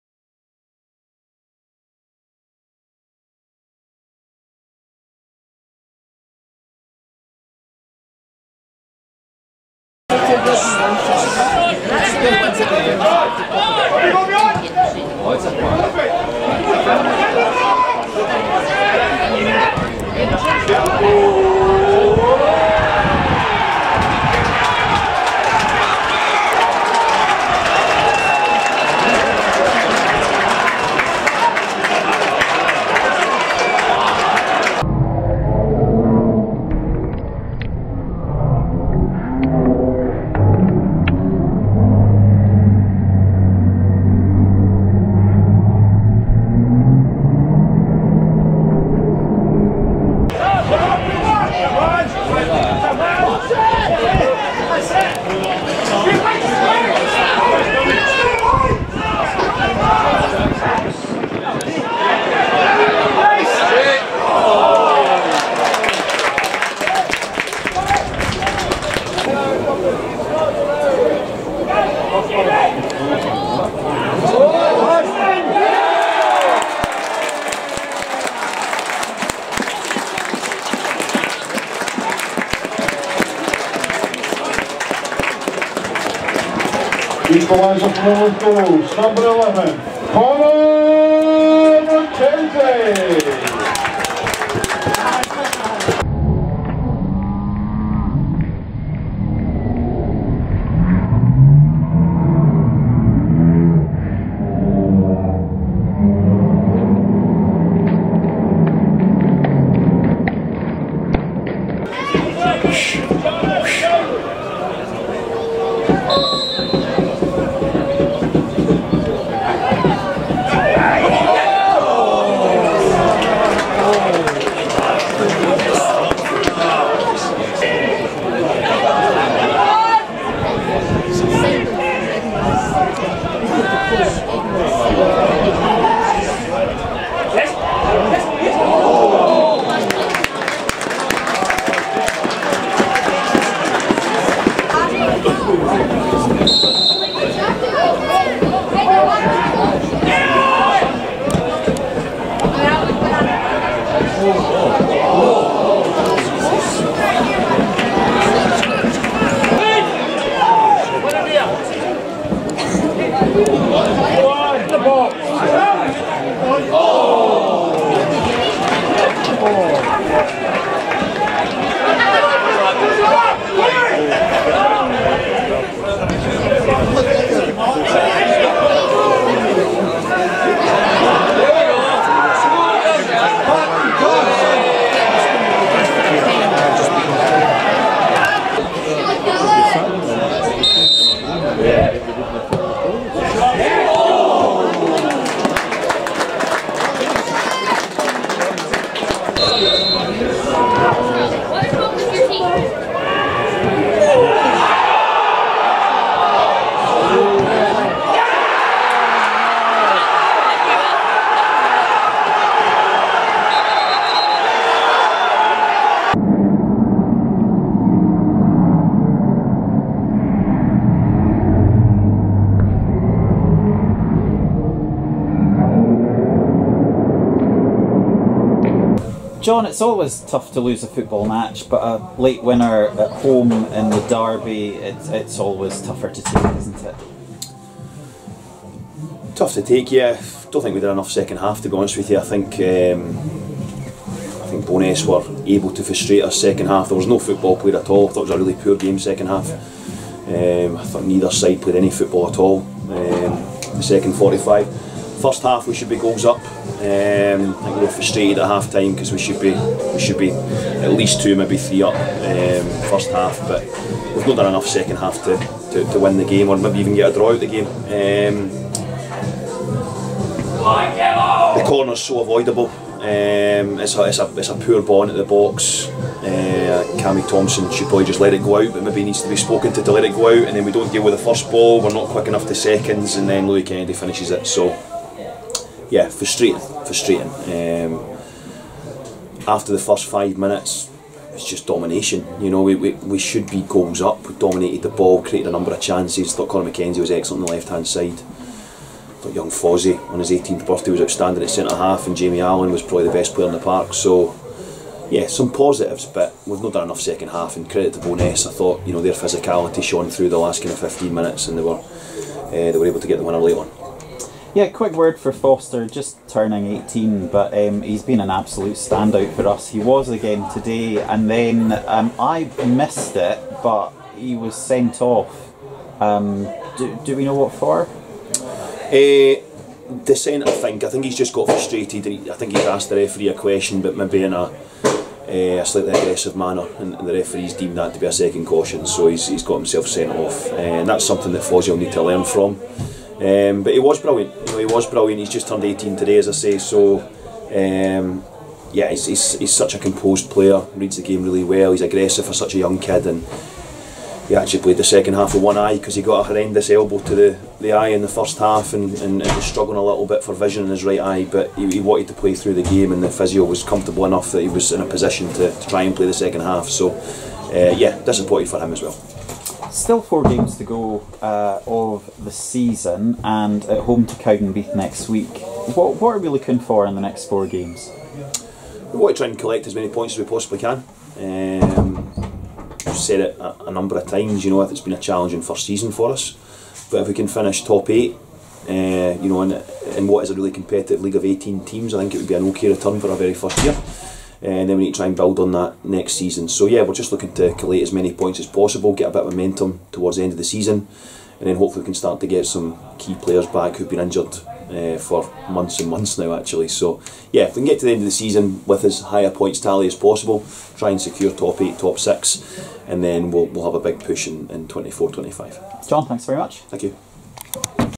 Linlithgow Rose v Bo'ness Utd 23 March 2024
Highlights from this Lowland League tussle at the MV Commercial Prestonfield Stadium as a goal from Conor McKenzie is not enough to stop the visitors claiming all three points in the Flints Derby. Stick around after the action for an exclusive post-match interview with Rose assistant boss John Millar.